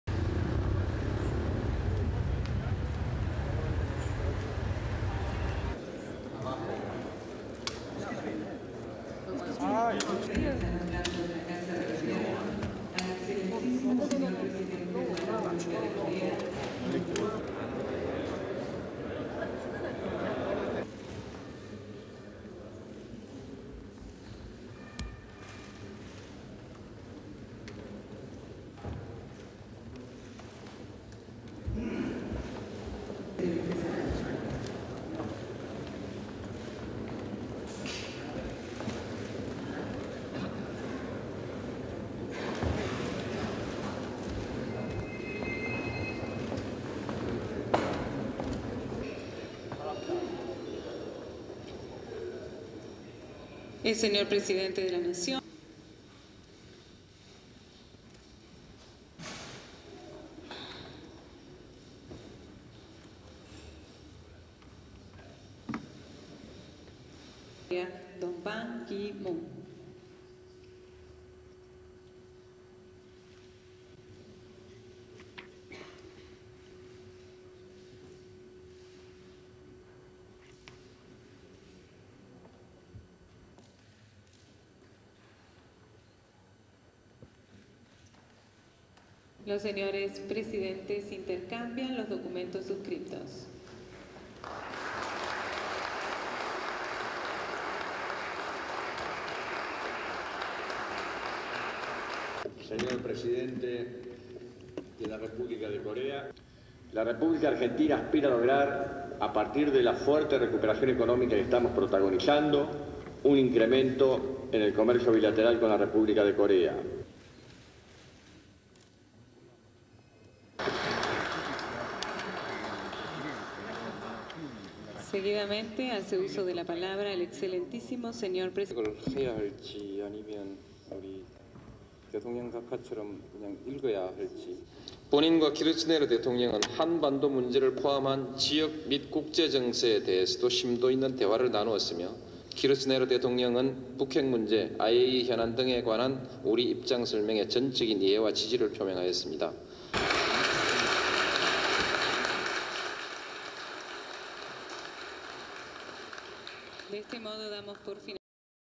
South Korean President Roh Moo-hyun visits Argentina
1. Wide shot exterior presidential "Casa Rosada" palace
2. Argentinian President Nestor Kirchner waiting for South Korean President Roh-Moo-Hyun
3. South Korean official delegation walking to greet President Kirchner
4. Close up of Roh-Moo-Hyun greeting Argentinian officials
5. South Korean officials greeting Kirchner
6. Argentinian officials greeting South Korean president
7. Wide shot of gathering
8. South Korean President Roh-Moo-Hyun and Argentinian president Nestor Kirchner sitting at table waiting to sign agreement
9. Argentinian foreign relations minister Rafael Bielsa signing document
10. South Korean foreign minister signing document
11. Mid shot of photographers
12. President Kirchner and Roh-Moo-Hyun exchanging signed agreement, attendees applauding
13. Wide shot of attendees listening to president Kirchner´s speech
14. SOUNDBITE: (Spanish) Nestor Kirchner, President of Argentina:
"The Argentinian Republic is willing to achieve, since we have experienced a strong economic recovery, economic cooperation with the republic of Korea. "
15. Photographer taking a picture of ceremony
16. Attendees applauding at the end of Kirchner´s speech, Kirchner walks away from podium
17. Roh-Moo-Hyun at podium giving his speech
18. SOUNDBITE: (Korean) Roh-Moo-Hyun, President of South Korea:
"President Kirchner and I have agreed to co-operate to solve in a peaceful way the issue of nuclear proliferation with North Korea. This will provide us with strong international support "
19. Wide shot of attendees applauding at the end of official ceremony, Kirchner and Roh-Moo-Hyun shaking hands

STORYLINE:

The presidents of Argentina and South Korea signed trade accords on Monday aimed at bolstering commercial ties that included a Korean offer of
30 (m) million US dollars in credit - the first in foreign funds since Argentina's 2001 debt default.

South Korean President Roh Moo-Hyn and President Nestor Kirchner signed a series of agreements intended to deepen economic and cultural links between the two countries.

The announcements came during a three-day visit to Argentina by the South Korean president, who arrived in Buenos Aires on Sunday.

The highlight of the agreements included a commitment from South Korea's Export Import Bank to extend 30 (m) million US dollars in credit to the Argentine government to buy South Korean products and contract services from Korean companies.

Argentine officials hailed the agreement as a sign the country was moving toward winning back investors after it stopped payment on more than 100 (b) billion dollars in public default during the height of the country's economic crisis.

The financial turmoil also saw the government freeze bank accounts and devalue the Argentine currency after days of deadly street riots and political unrest.

Argentina's Planning Minister Julio De Vido described the Korean credit offer "as a significant gesture" since the country is still struggling to restructure its debt and reach agreement with its creditors.

Argentine authorities have been wrangling with bondholders for more than a year over repayment terms.

After the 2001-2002 meltdown, Argentina's economy began growing again last year and the government forecasts economic growth will top seven percent this year.